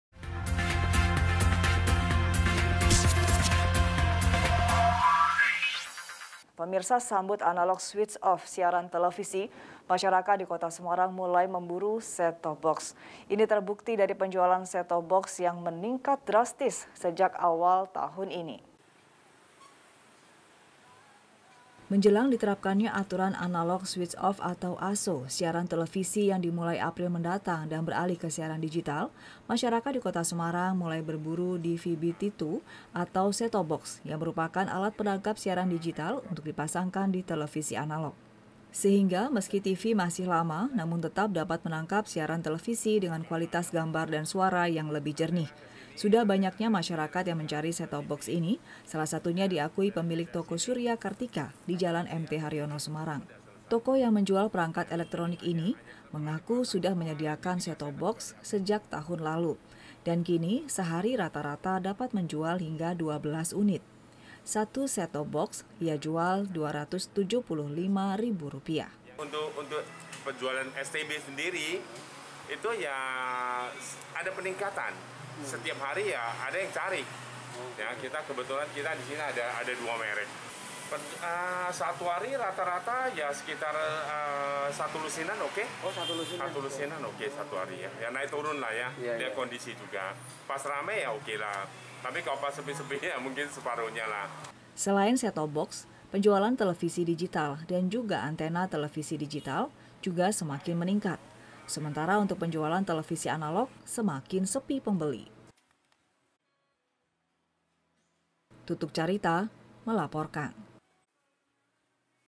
Set Top Box Mulai Diburu Masyarakat, Penjualan Naik Drastis
Semarang - Sambut analog switch off siaran televise, masyarakat di Kota Semarang mulai memburu set top box. Ini terbukti dari penjualan set top box yang meningkat drastis sejak awal tahun ini. (tuk)